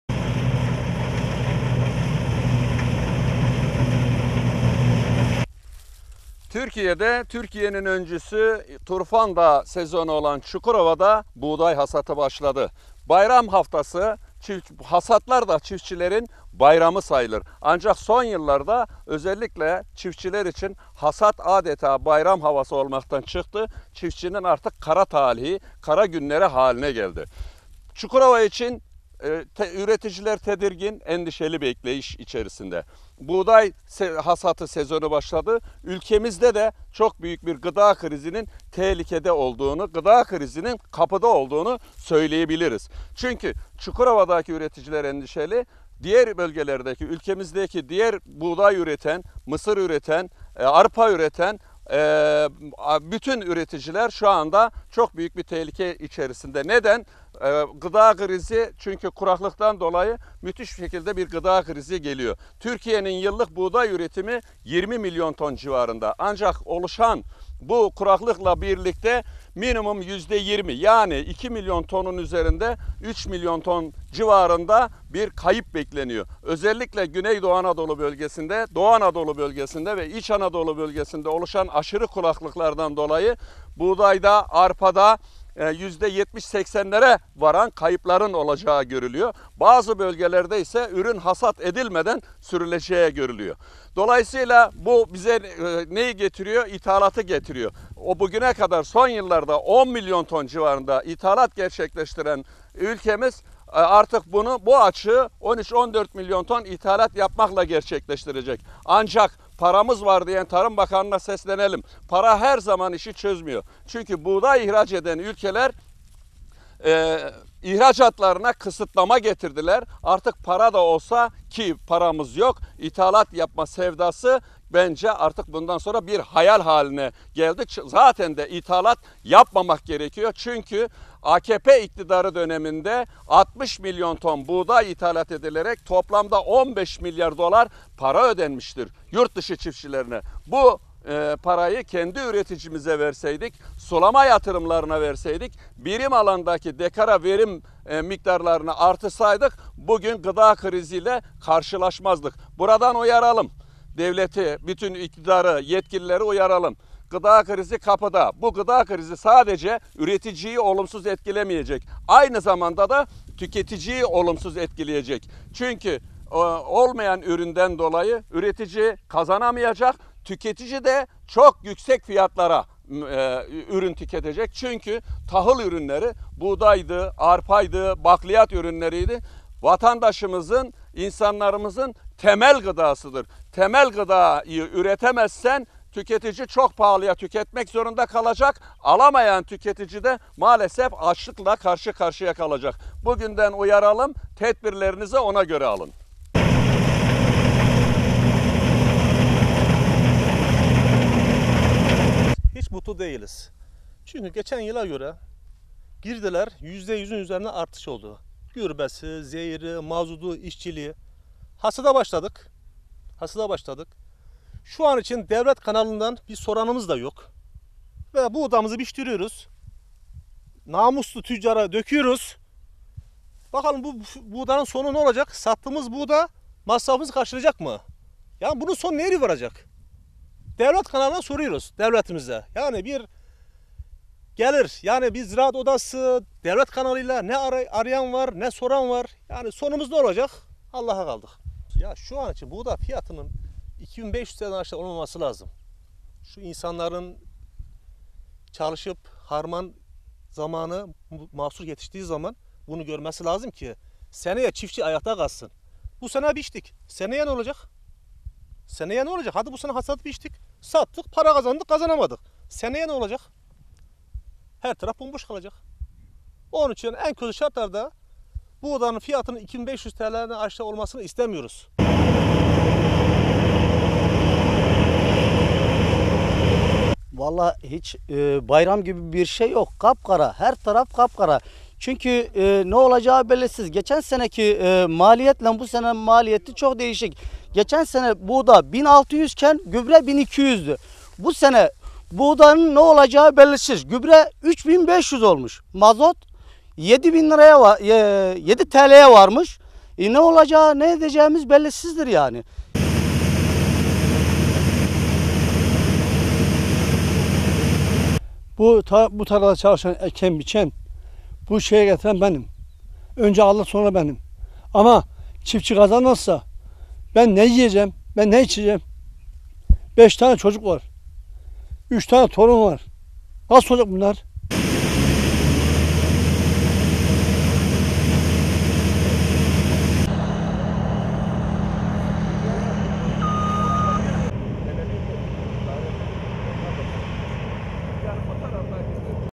Barut; Gıda krizi kapıda, acil önlem alınmalı, çözüm bulunmalı
Cumhuriyet Halk Partisi (CHP) Adana Milletvekili, TBMM Tarım, Orman ve Köyişleri Komisyonu Üyesi Ayhan Barut, Çukurova’da başlayan buğday hasadına katıldı, kuraklık nedeniyle yaklaşan gıda krizine karşı iktidarı göreve çağırdı.